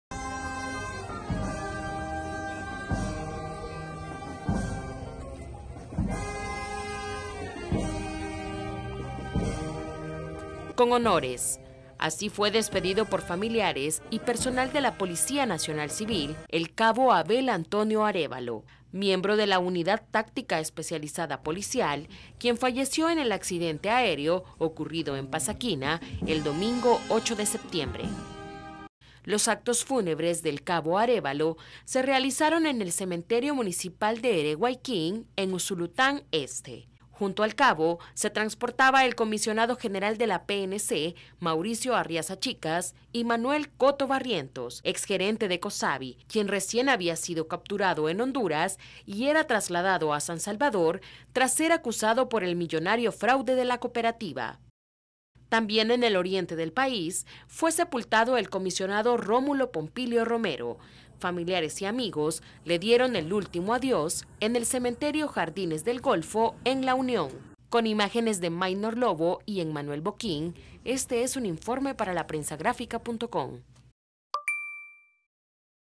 Despiden al cabo de la PNC Antonio Arévalo fallecido en el accidente aéreo
Los actos fúnebres del cabo Arévalo se realizaron en el cementerio municipal de Ereguayquín, en Usulután Este.